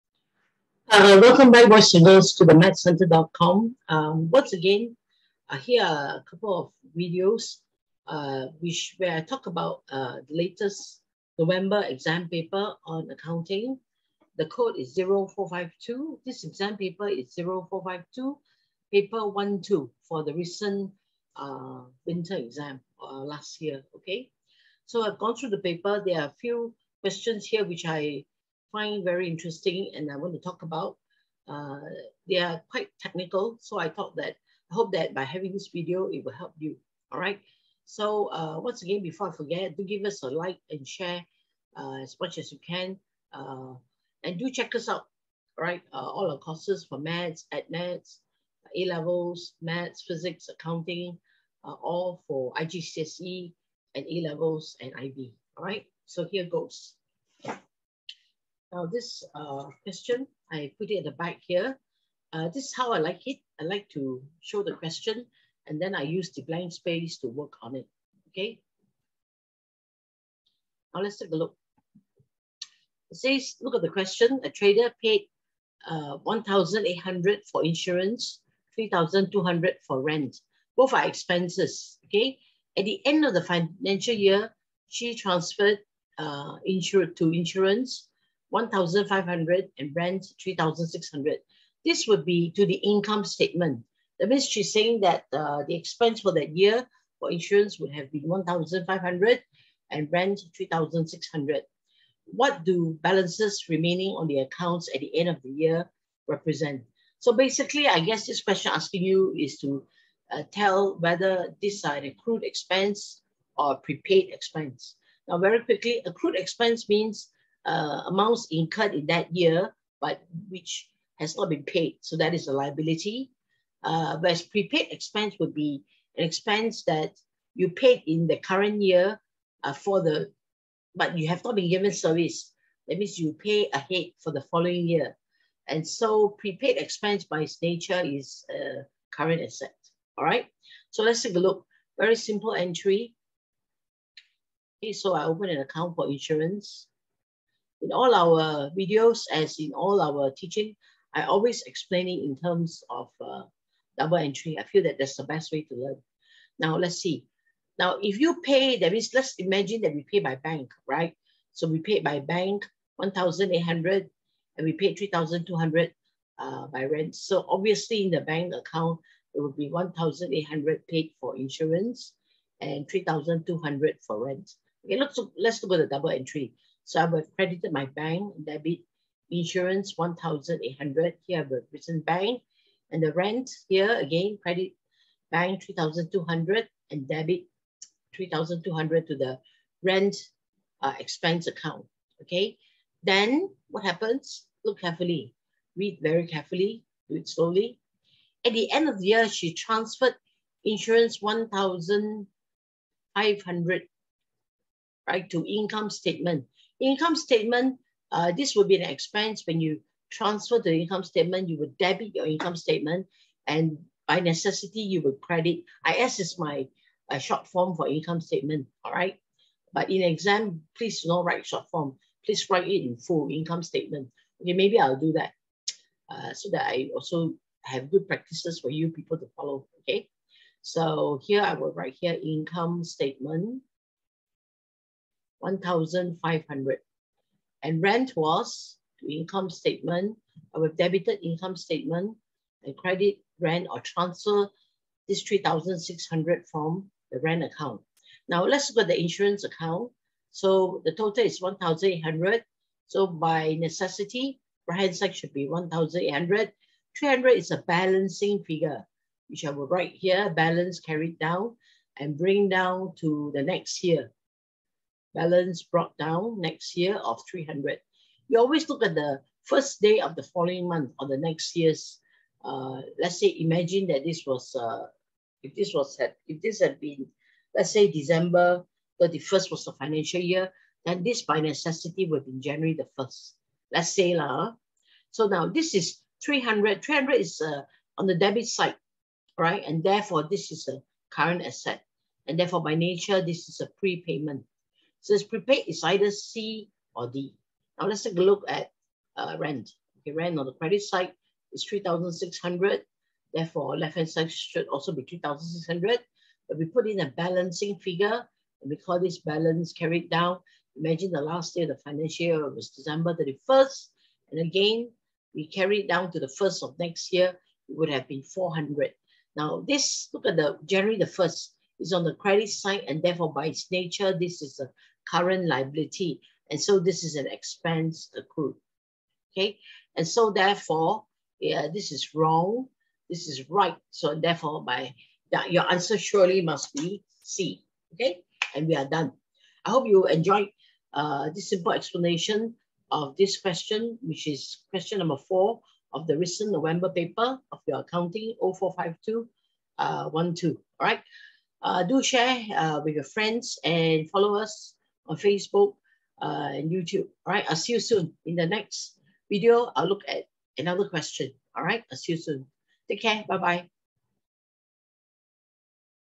Cambridge IGCSE Accounting (0452/12) Oct/Nov 2021 Exam Solutions Q4
Cambridge IGCSE Accounting (0452/12) Oct/Nov 2021 Exam Question 4 fully worked solutions video.

Follow us to get notification of our next video.

Also check out our FREE IGCSE Add Maths (0606) Year 10 Special Intro Course !